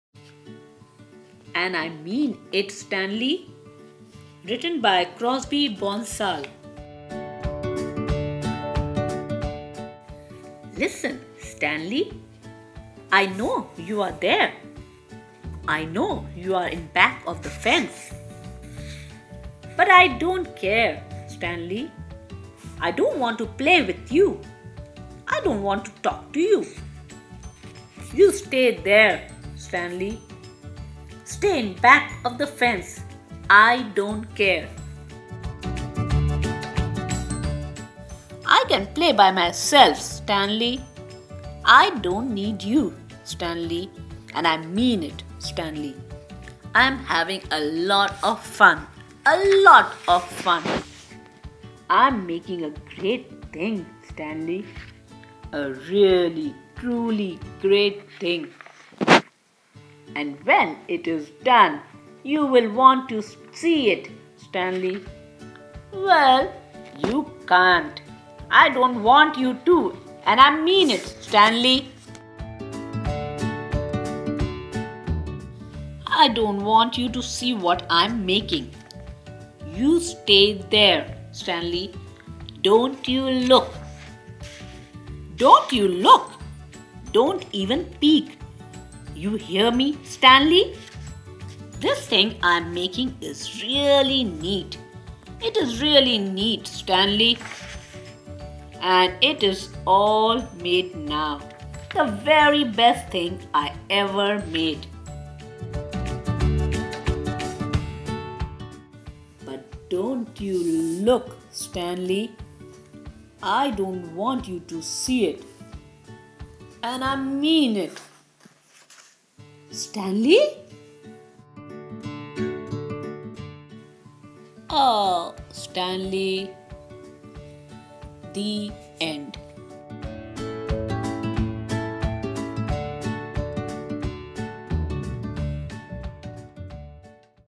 📚 AND I MEAN IT, STANLEY by Crosby Bonsall | Kids Read Aloud | Story Time
In "And I Mean It, Stanley" by Crosby Bonsall, Stanley the dog is determined to prove he can be helpful, but his good intentions keep causing trouble! This charming and funny story teaches kids about responsibility, persistence, and the importance of trying your best.

Written by Crosby Bonsall.